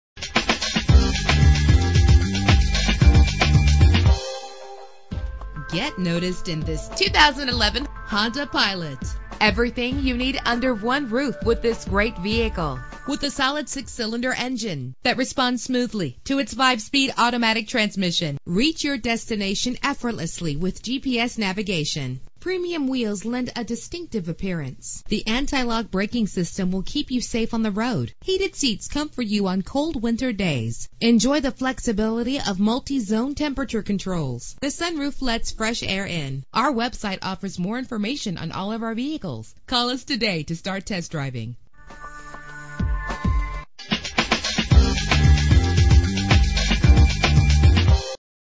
2011 Honda Pilot - Laurel MD
Year: 2011
Make: Honda
Model: Pilot
Trim: Touring w/ RES/ Navigation
Engine: 3.5 liter 6 cylinder 24 valve
Transmission: 5-Speed Automatic
Color: Blue
Mileage: 5
Address:
3371 Laurel Fort Meade Road